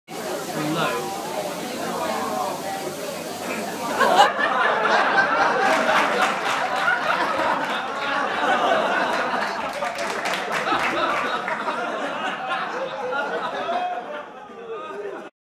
2013 J&S blow out their B'day Cake
Birthday cake candle blowing - difficult for some